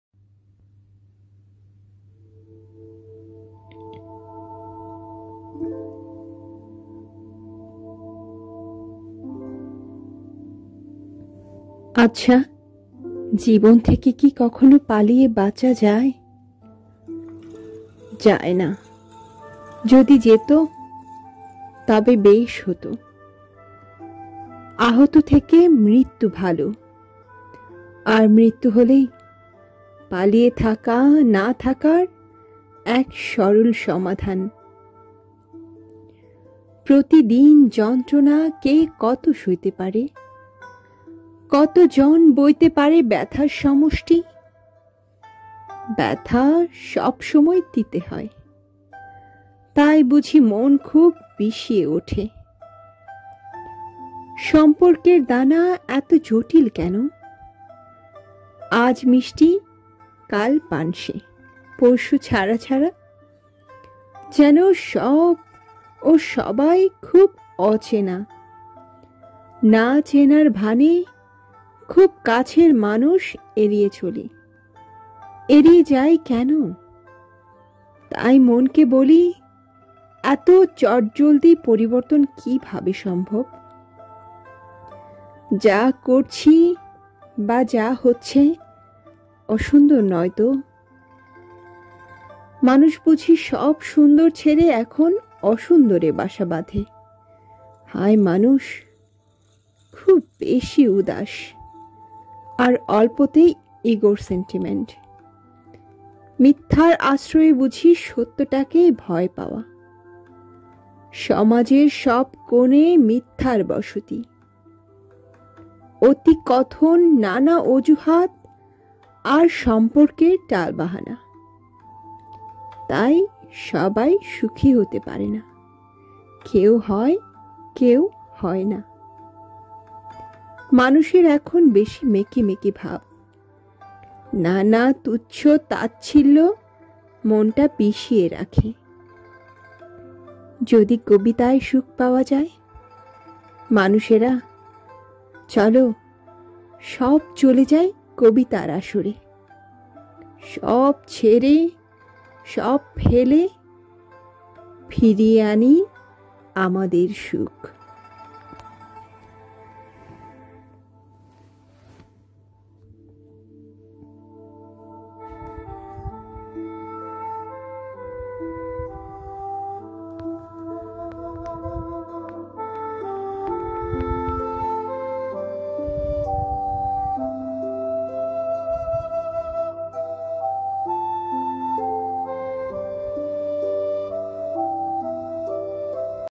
বাংলা কবিতা আবৃত্তি
বাংলা কবিতা
সবাই সুখী হতে পারে না
কলমে: দেবদুলাল বণিক
কন্ঠ: রুমা জেকলিনা কস্তা